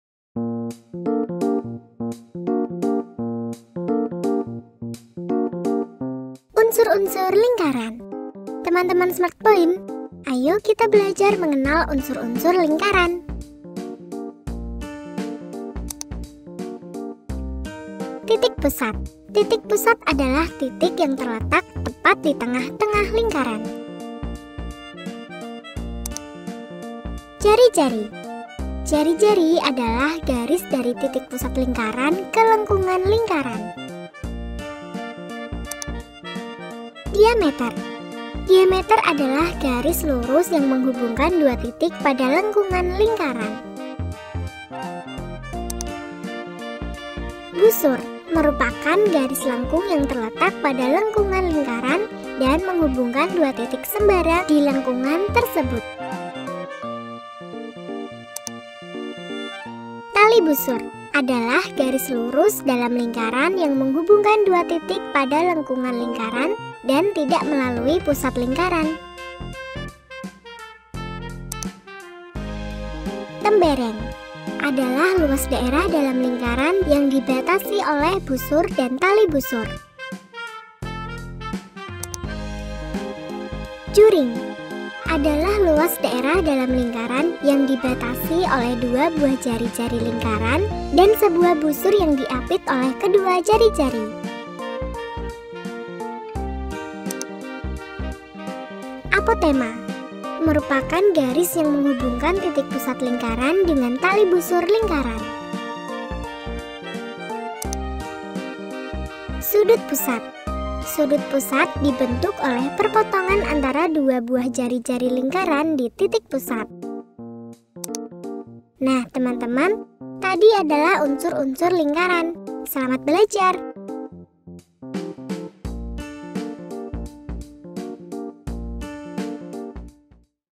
Unsur-unsur Lingkaran (SD6005TDP)
Teman Smartpoint, kali ini kita akan belajar mengenal apa itu diameter, tali busur, tembereng, dan lainnya. SImak video ini selengkapnya